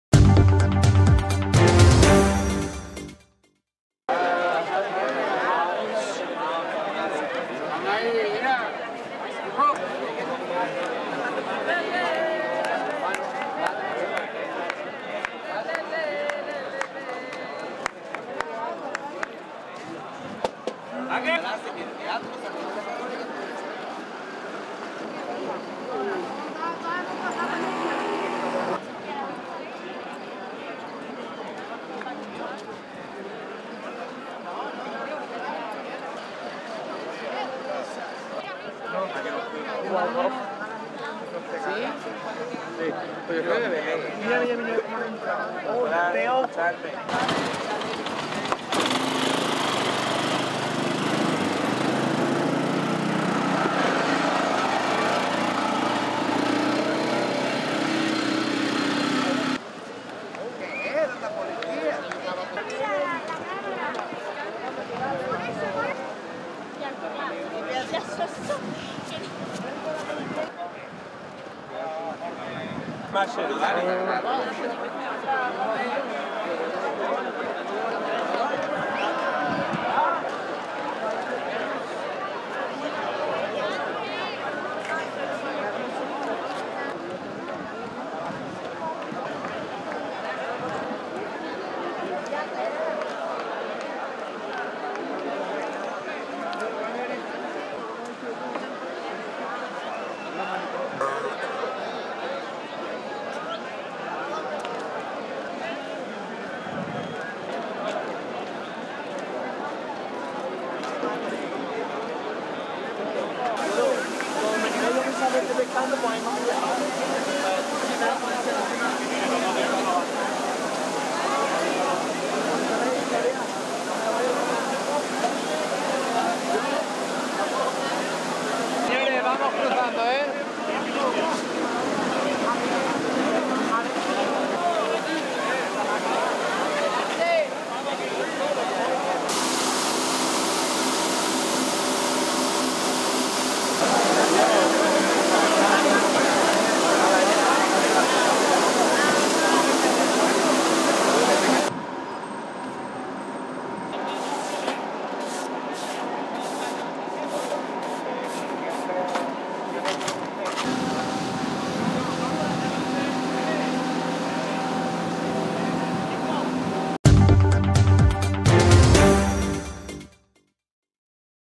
Desalojo y limpieza de La Playa Victoria
Efectivos de Policia Local, Guardia Civil y Policia nacional desalojaron la playa sin ningún incidente y dejaron paso a los equipos de limpieza.